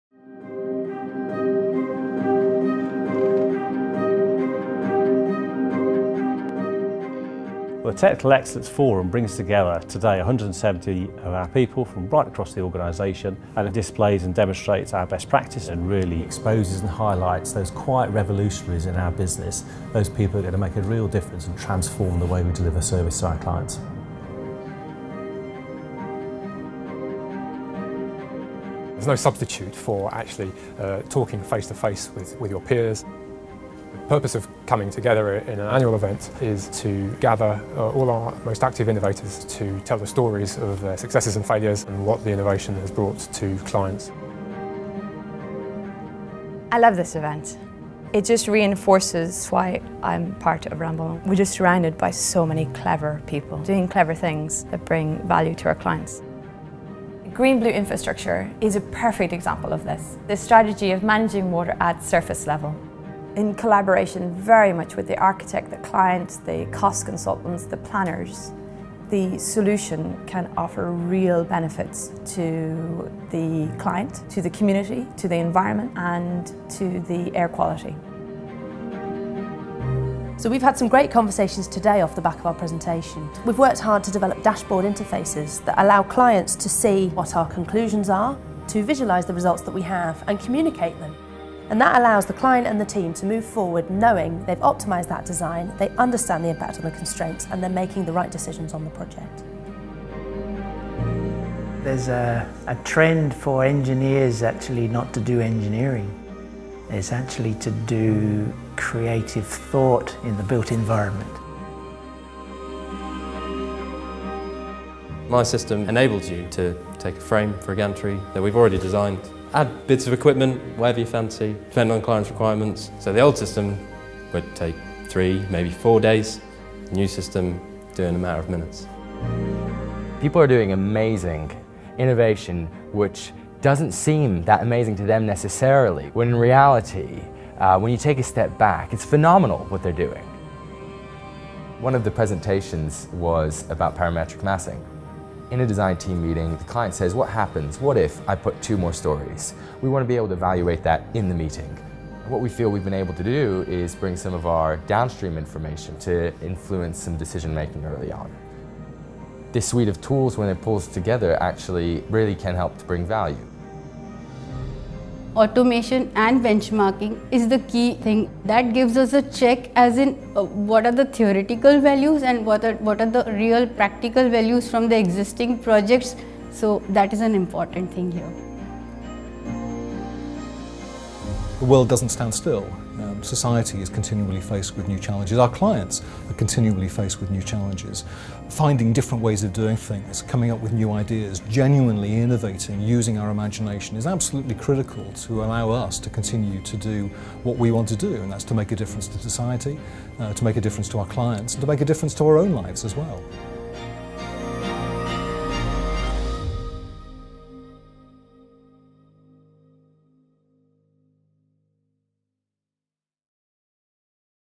Ramboll TEF 2017 - Sharing innovation and best practice
This short video provides insight into our annual Technical Excellence Forum (TEF), where Ramboll's quiet revolutionaries gather to share recent innovations and the value these have created for our clients.
Introduced by Ramboll UK MD Mathew Riley, we capture the thoughts of some of Ramboll's leading engineers on the need to innovate, learn and share.